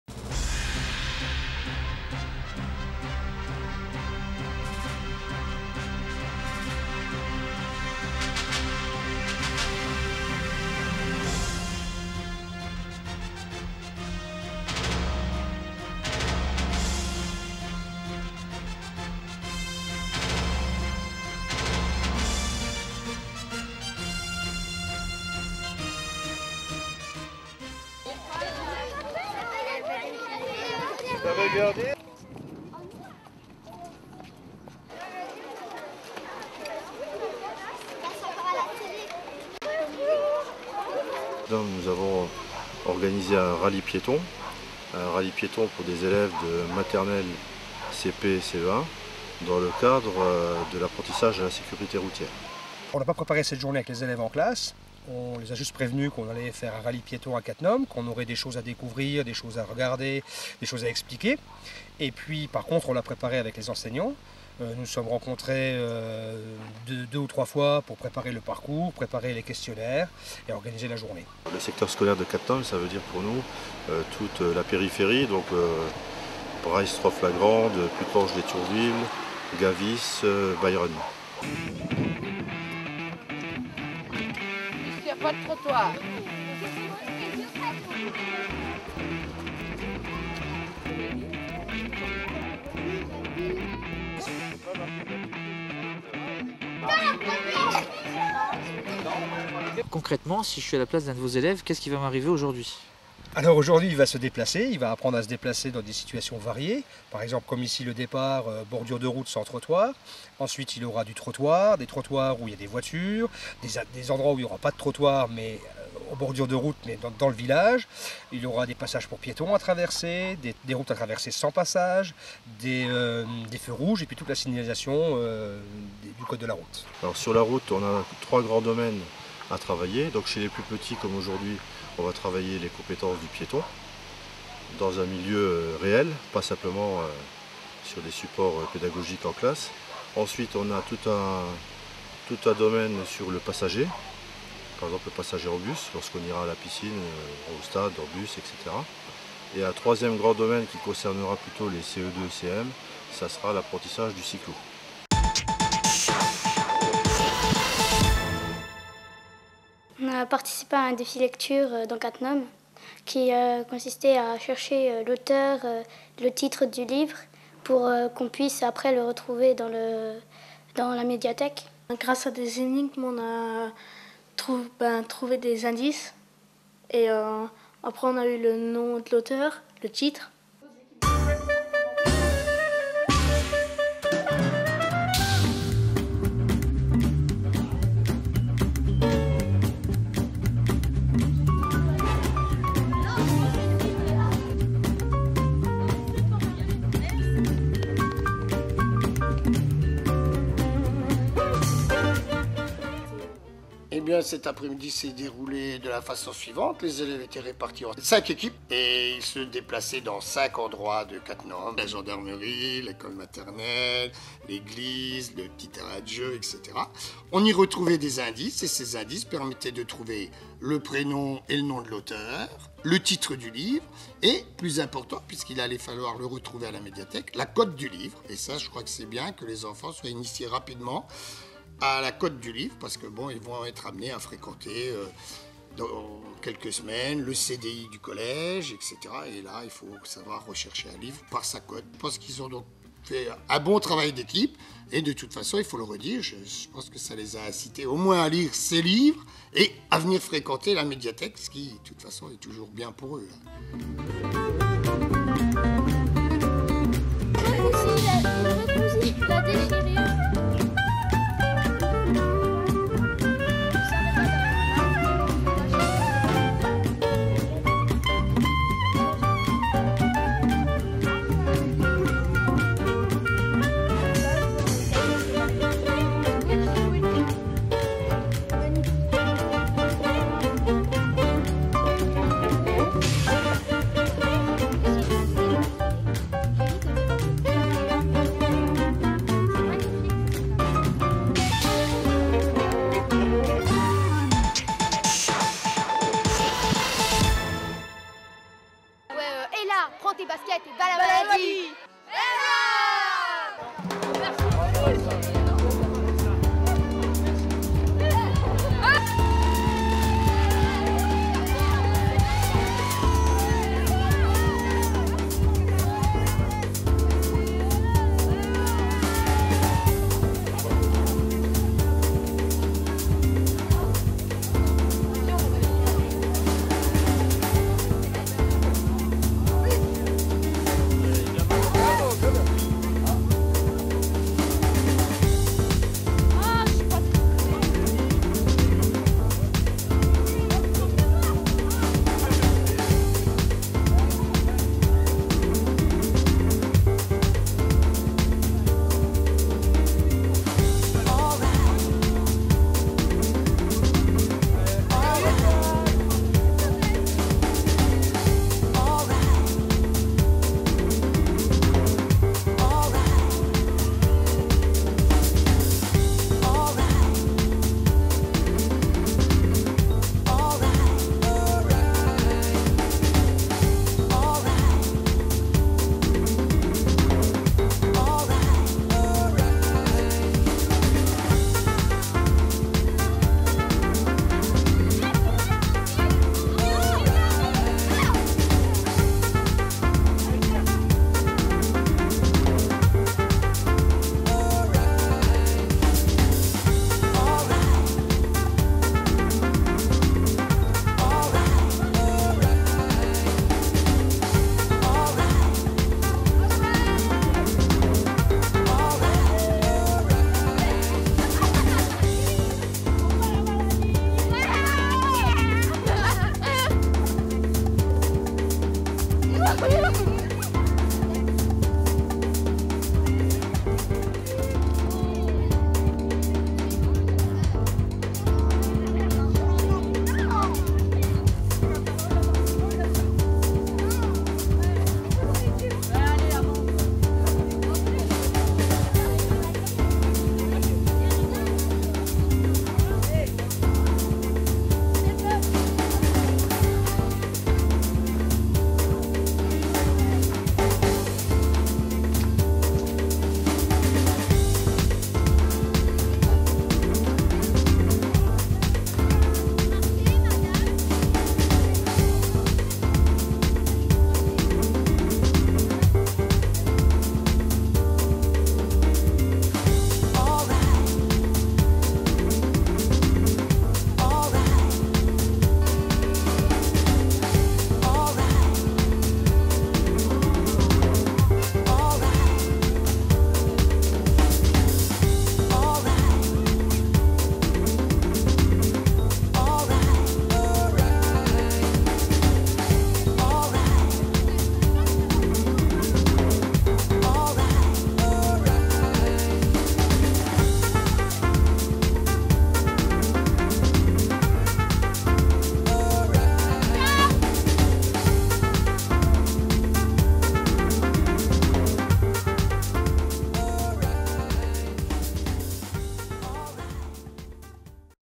jt cattenom reflets 03 08 2009
Permis Piéton avec les Ecoles des environs de Cattenom, insolivres 2009 avec la médiathèque, marche ELA avec le collège de Cattenom